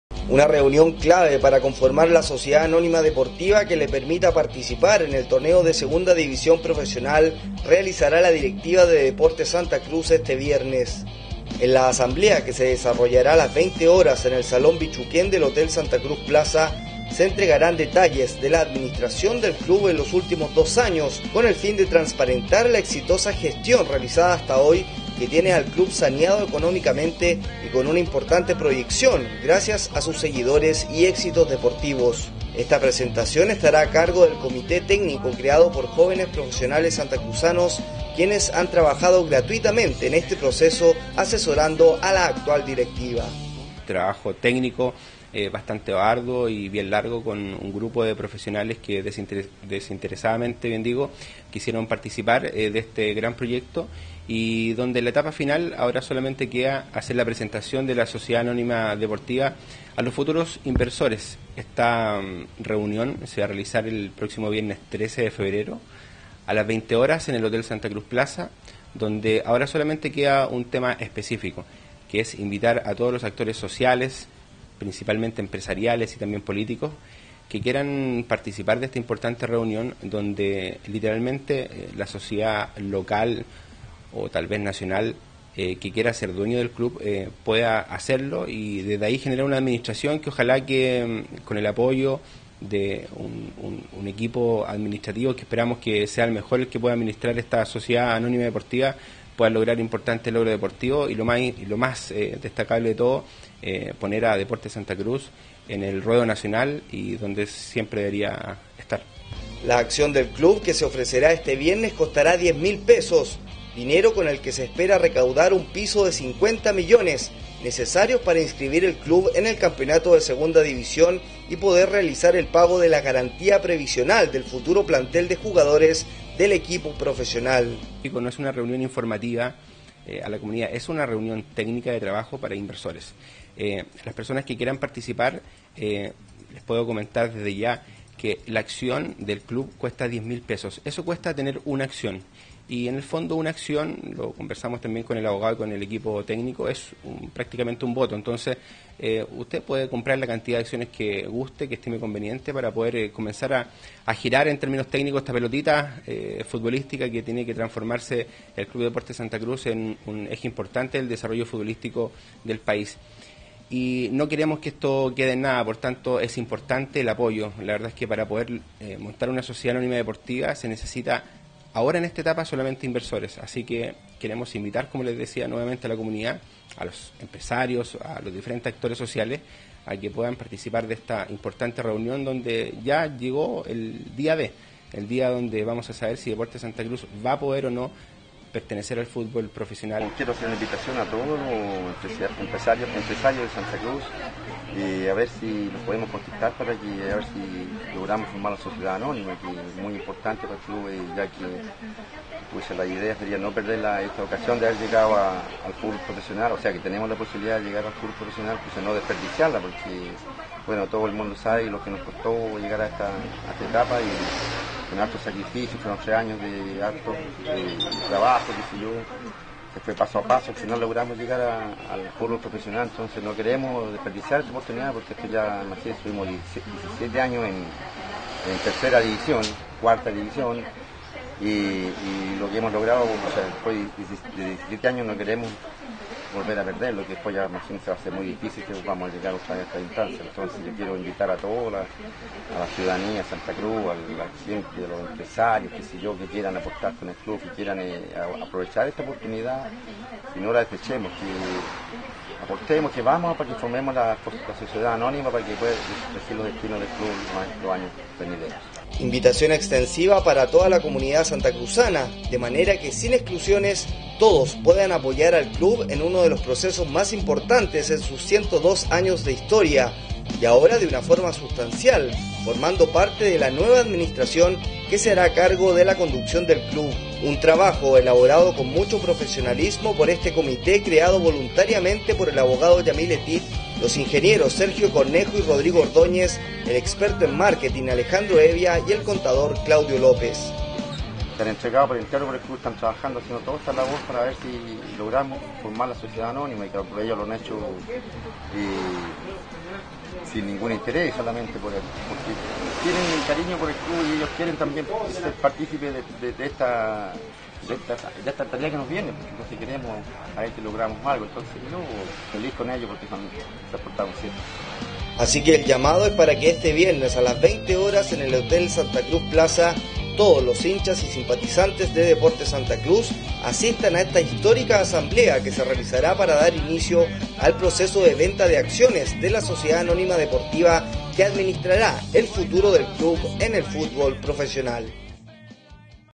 SADP Deportes Santa Cruz
Este viernes 13 de febrero se decidirá el futuro profesional de Deportes Santa Cruz en la reunión que se realizará en el Hotel Santa Cruz Plaza, ya que se pondrán a la venta las acciones de la SADP que permitirá al club participar en el torneo de Segunda División.